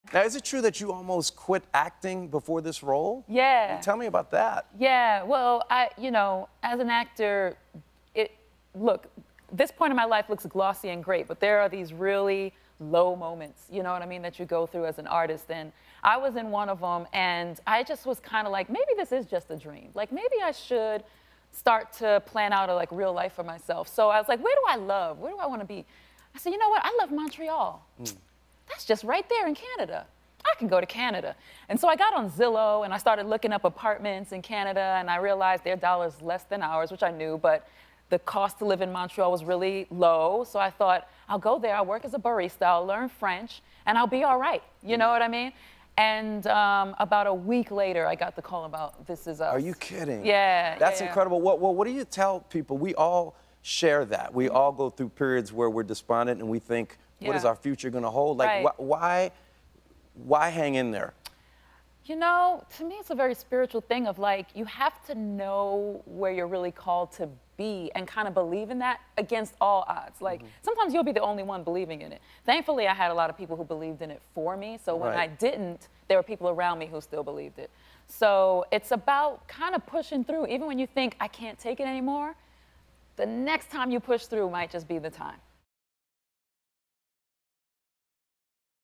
"This Is Us" star Susan Kelechi Watson Almost Quit Acting
Susan Kelechi Watson almost quit acting right before hit series "This Is Us" called her!